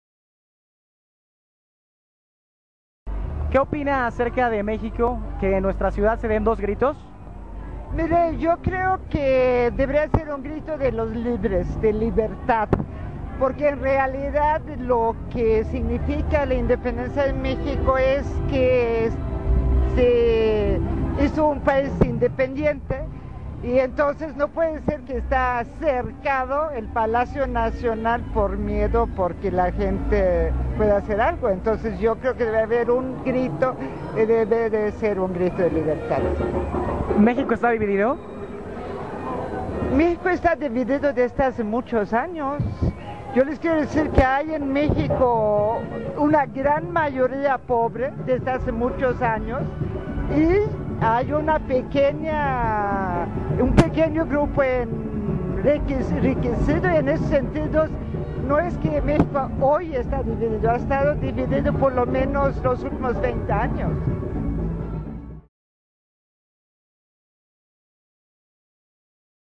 15 de Septiembre, Entrevista Ana Cristina Luarel
Cobertura de la Celebración realizada en el Zócalo de la Ciudad de México en el Aniversario 198 de la Independencia. Una dama da su punto de vista sobre los acontecimientos que vive el país.